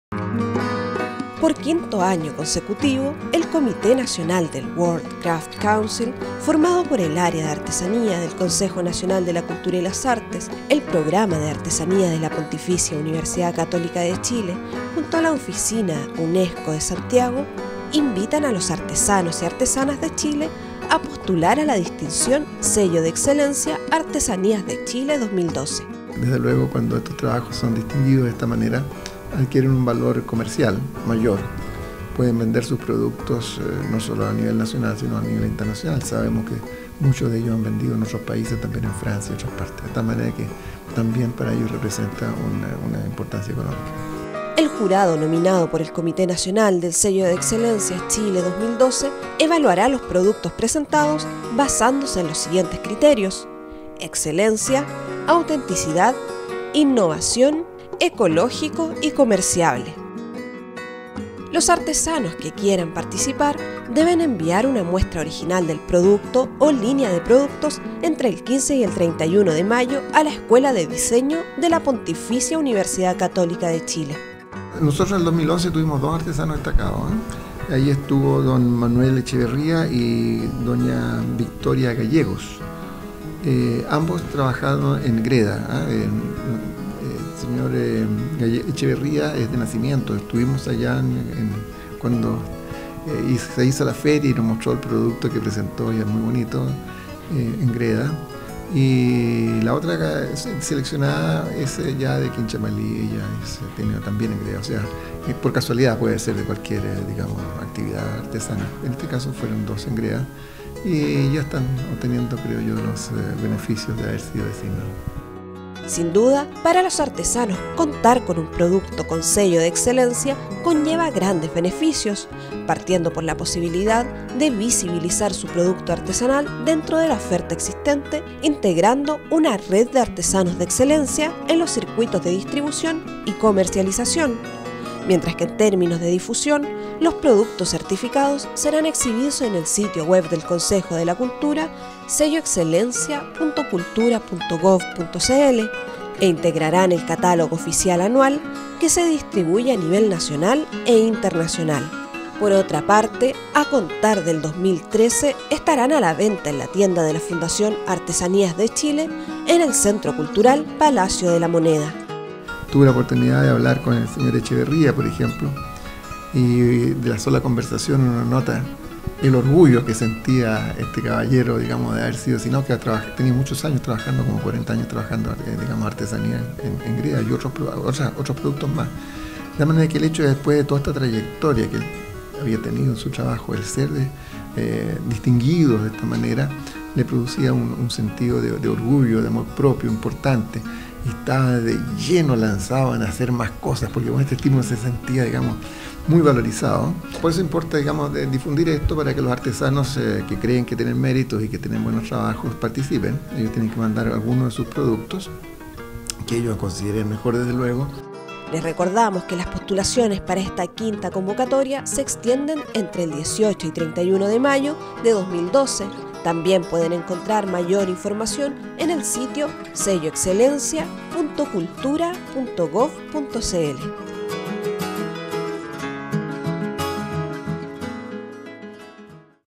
Convocatoria Sello de Excelencia Artesanía 2012
Ya está abierta la convocatoria a los artesanos de la región para postular al Sello de Excelencia en Artesanía 2012.